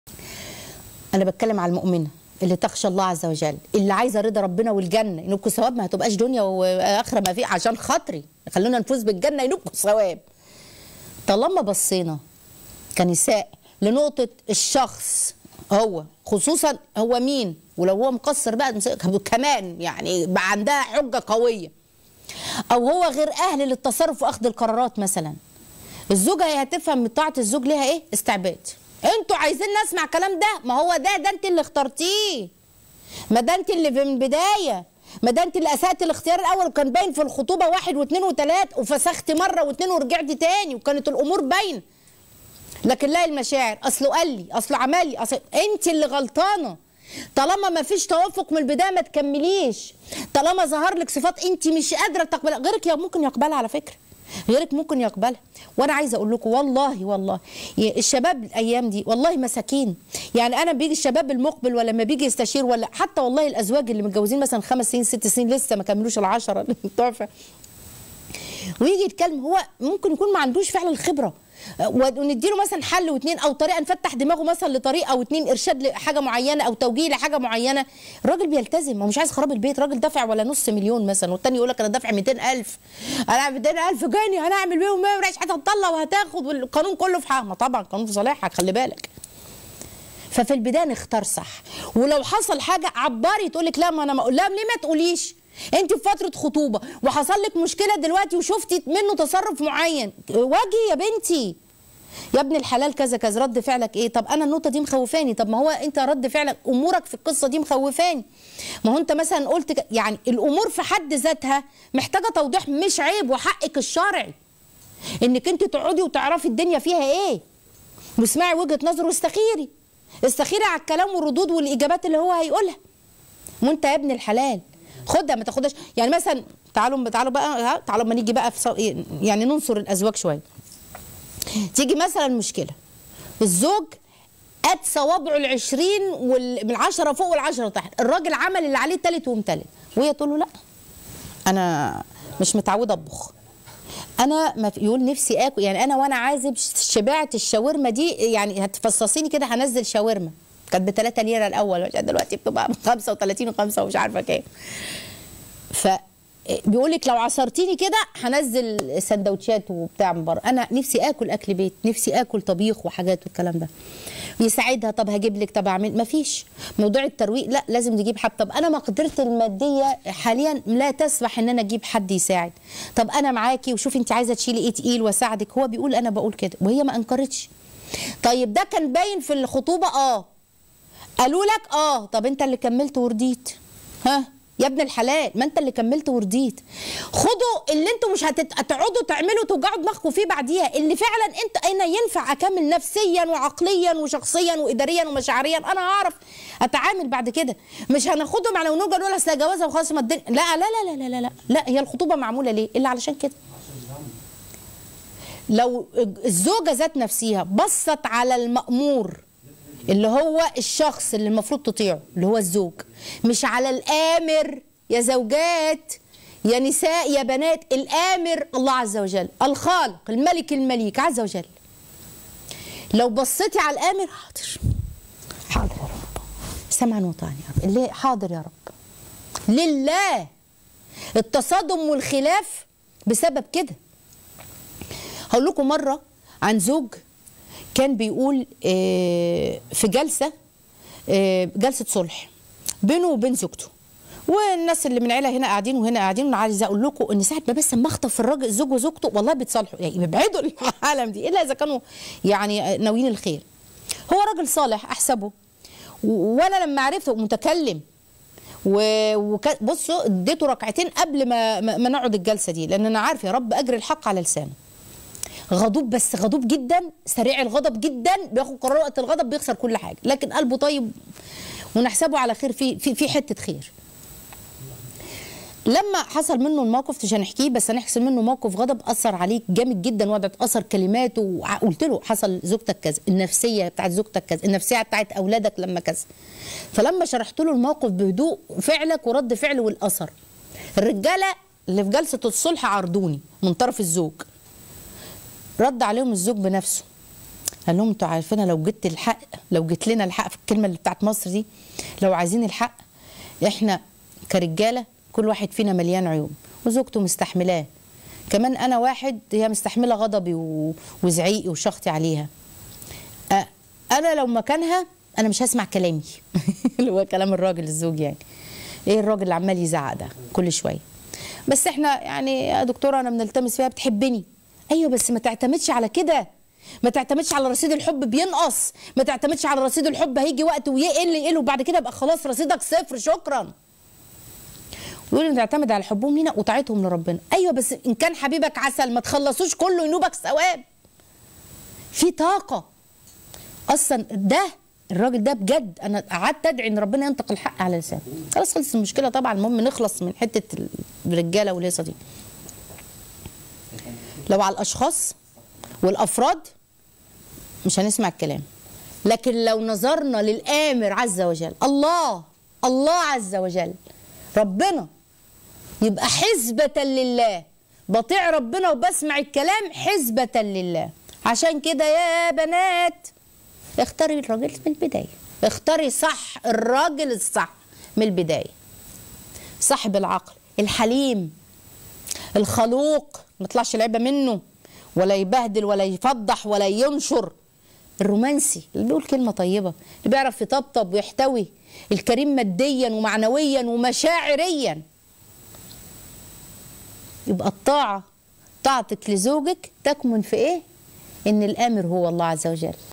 وصية من ذهب من د. هالة سمير للمقبلين على الزواج.. متكمليش ومتكملش!!
حلقة بعنوان| طاعة الزوجة لزوجها

 shorturl.at/afwD9